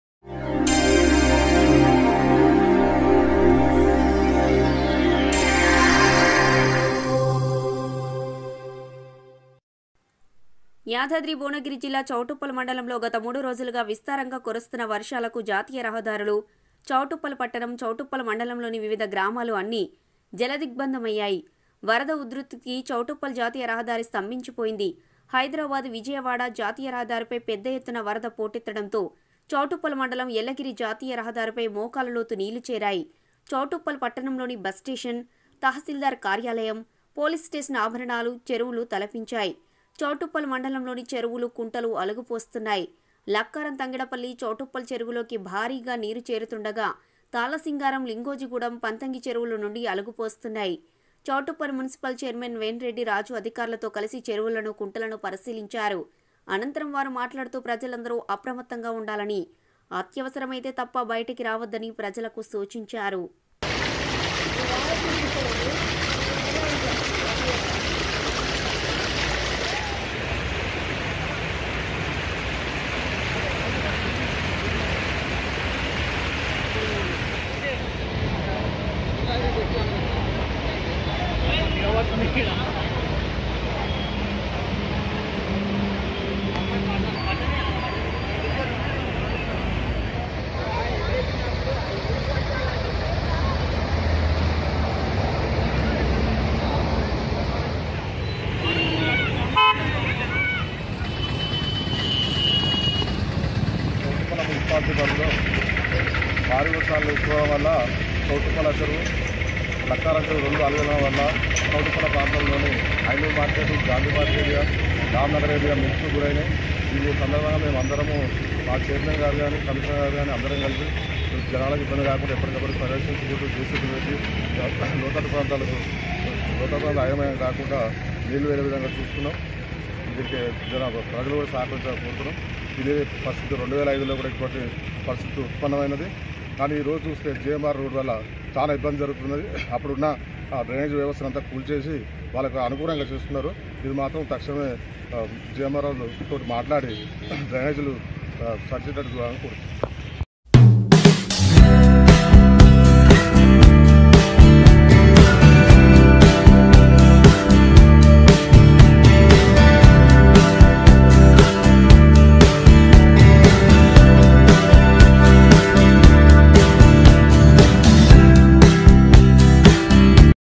YADADHRI||Choutuppal mandal 3rojulaga kurise varshamvalla pradhana rahadharulu Jeladhigbandham iyaye
యాదాద్రి భువనగిరి జిల్లా చౌటుప్పల్ మండలం లో గత మూడు రోజులుగా విస్తారంగా కురుస్తున్న వర్షాలకు జాతీయ రహదారులు, చౌటుప్పల్ పట్టణం, చౌటుప్పల్ మండలం లోని వివిధ గ్రామాలు అన్ని జల దిగ్బంధం అయ్యాయి వరద ఉధృతికి కి చౌటుప్పల్ జాతీయ రహదారి స్తంభించిపోయింది. హైదరాబాద్ విజయవాడ అ జాతీయ రహదారిపై పెద్ద ఎత్తున వరద పోటెత్తడంతో చౌటుప్పల్ మండలం ఎల్లగిరి జాతీయ రహదారిపై మోకాలు లోతు వీళ్లు చేరాయి చౌటుప్పల్ పట్టణంలోని బస్ స్టేషన్, తాసిల్దార్ కార్యాలయం, పోలీస్ స్టేషన్ ఆభరణాలు చెరువును తలపించాయి. చౌటుప్పల్ మండలం లోని చెరువులు కుంటలు అలుగు పోస్తున్నాయి. లక్కారం, తంగెడ పల్లి, చౌటుప్పల్ చెరువులోకి భారీగా మీరు చేరుతుండగా తాళ్ల సింగారం, లింగోజిగూడెం, పంతంగి చెరువులు నుండి అలుగు పోస్తున్నాయి చౌటుప్పల్ మున్సిపల్ చైర్మన్ వెన్ రెడ్డి రాజు అధికారులతో కలిసి చెరువులను ,కుంటలను పరిశీలించారు అనంతరం వారు మాట్లాడుతూ ప్రజలందరూ అప్రమత్తంగా ఉండాలని, అత్యవసరమైతే తప్ప బయటికి రావద్దని ప్రజలకు సూచించారు